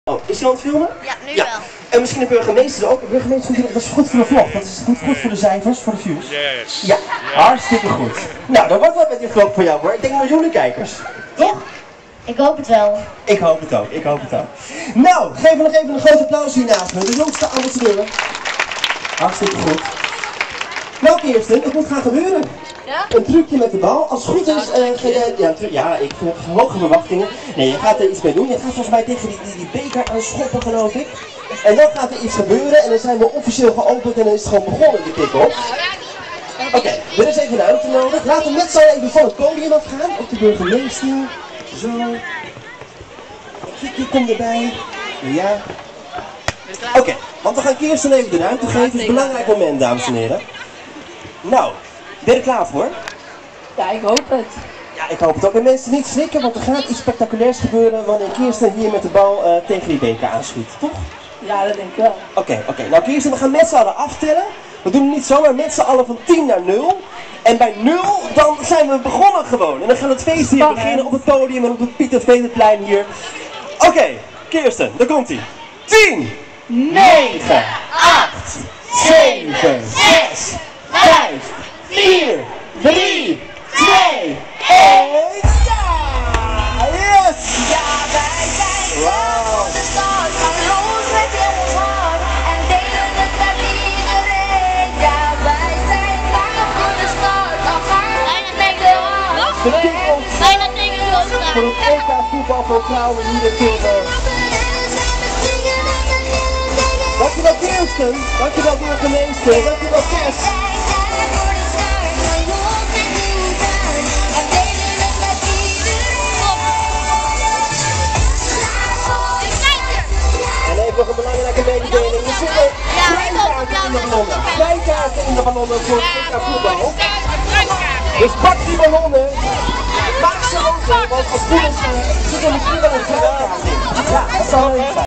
de buurgemeester van Tilburg intervieuwen voor het EK
weer moest ik op het podium komen om intervieuwd te worden, want toen het EK eenmaal bezig was, werd ik heeeel veel geintervieuwd (omdat ik jeugd ambassadeur ben). EN toen mocht ik ook de burgemeester intervieuwen. CIAO ADIOS AMIGOS